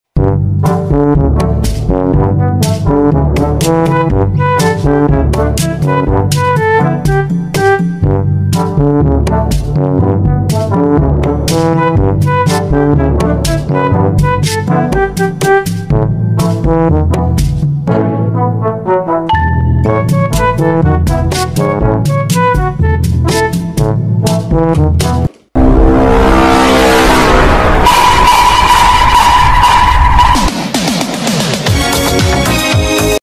How to Solve a Rubik's Cube UNDER 1 Second Trick!!
Rubik's Cube Magic Trick EXPOSED!!

My editing softwares (all for mobile devices)
Text to speed- Narrator- Text to speed- (tts)

Did everyone enjoy the video? Hope you all have a good day.